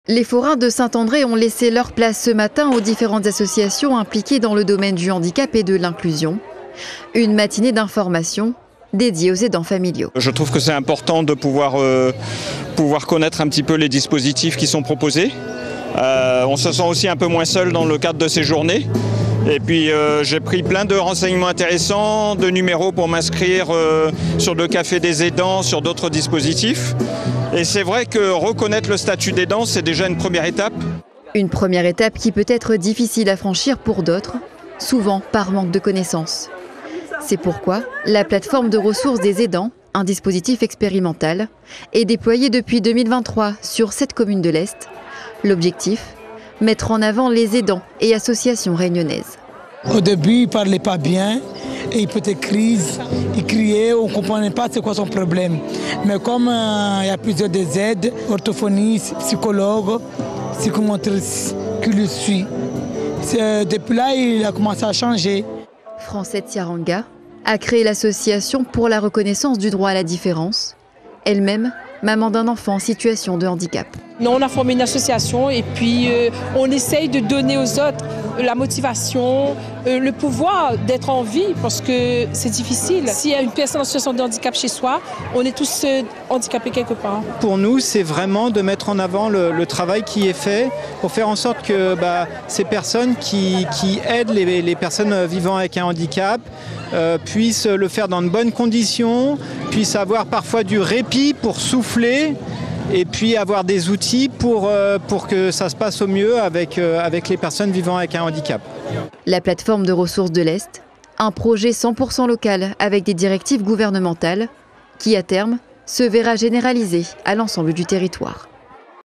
Une matinée festive pour faire connaître le travail des aidants familiaux
Une matinée festive pour faire connaître le travail des aidants familiaux ? C'était à Saint-André ce samedi.